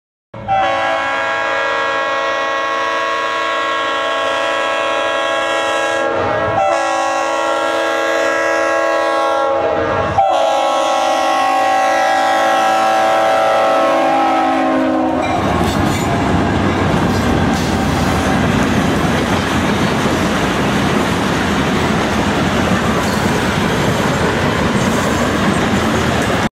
Northern System SD50 passes Though Iowa City, Iowa
Trains:
Northern System 5248 SD50 (OLS Unit)
Northern System 9268 GP38-2 (ex Green Ash Pacific)
Atlanta Lines 2097 GP40